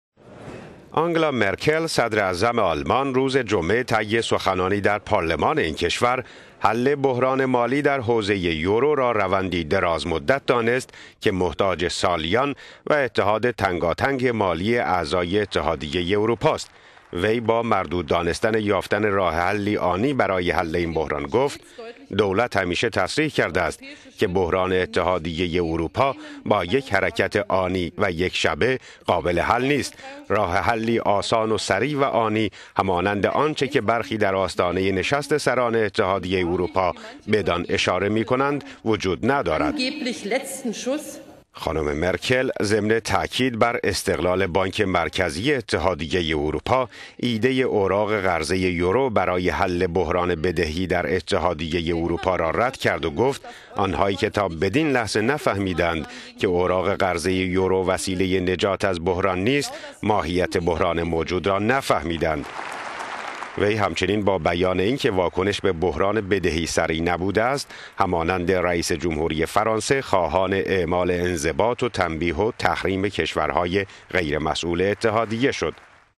مرکل: راه حلی آنی برای برون رفت از بحران یورو وجود ندارد
آنگلا مرکل صدر اعظم آلمان، روز جمعه (2 دسامبر) طی سخنانی در پارلمان این کشور، حل بحران مالی در حوزه یورو را روندی درازمدت دانست که محتاج سالیان و اتحاد تنگاتنگ مالی اعضای اتحادیه اروپا است.